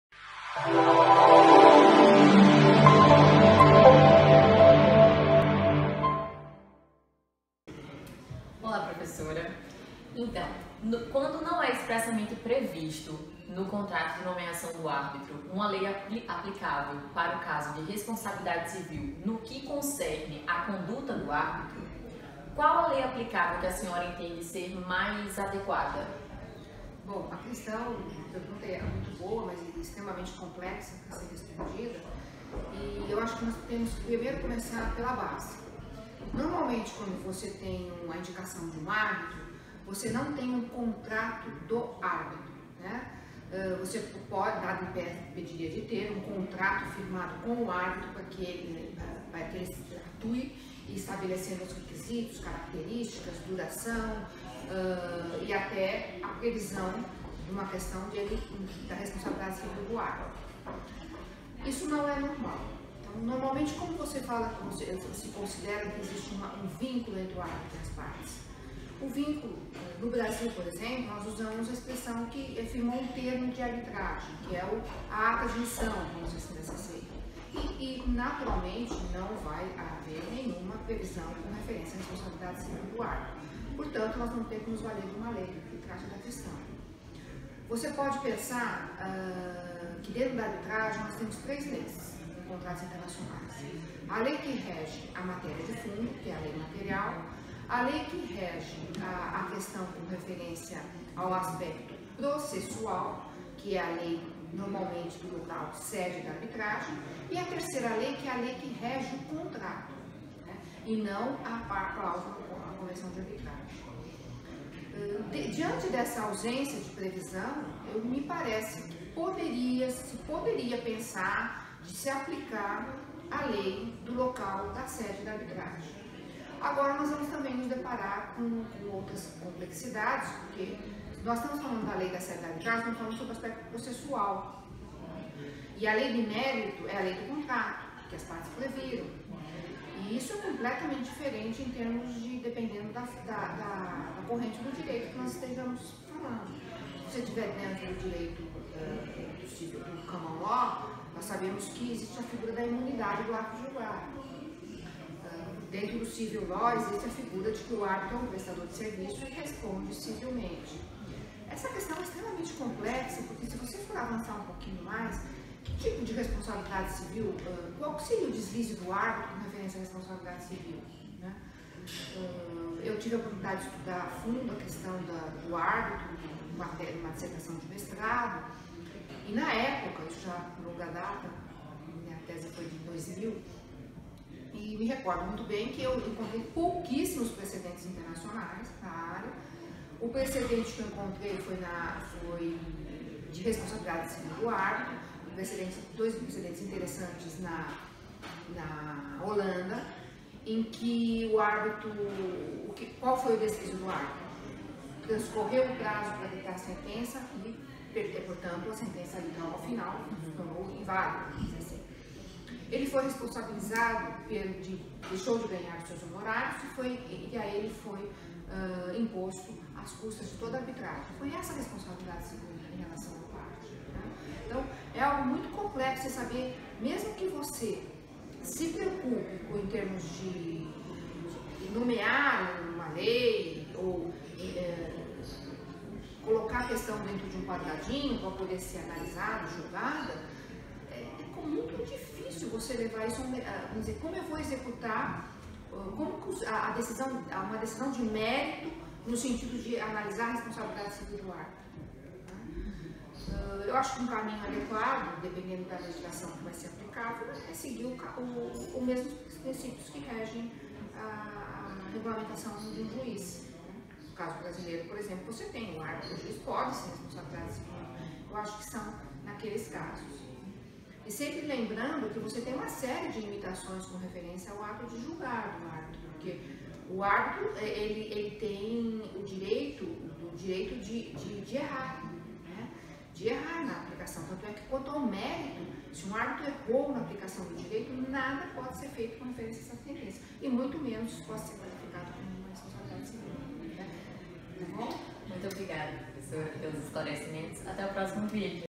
NELB Convida | Primeira Temporada - Programa 32
Nesta edição do NELB CONVIDA apresentamos o debate sobre: “Lei aplicável para casos de responsabilidade civil no que concerne a conduta do árbitro” com a Professora Doutora Selma Lemes da Universidade de São Paulo e a diretora do NELB e Mestranda Carmille Leal.

“É uma questão bem complicada, mas acredito que seguir as regras de responsabilidade civil aplicáveis ao juiz é uma saída”.